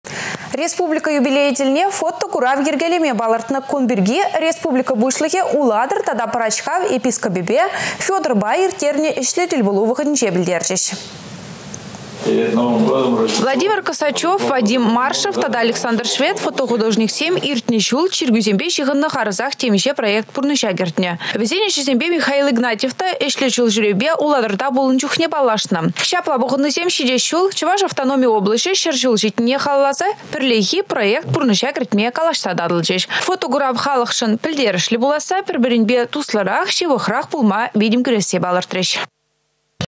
Пĕрлехи проект
Республика юбилейĕ тĕлне фотокурав йĕркелеме палăртнă. Кун пирки Республика Пуçлăхĕ Улатăр тата Пăрачкав епископĕпе Федорпа ирттернĕ  ĕçлĕ тлпулу вăхăтĕнче пĕлтерчĕç.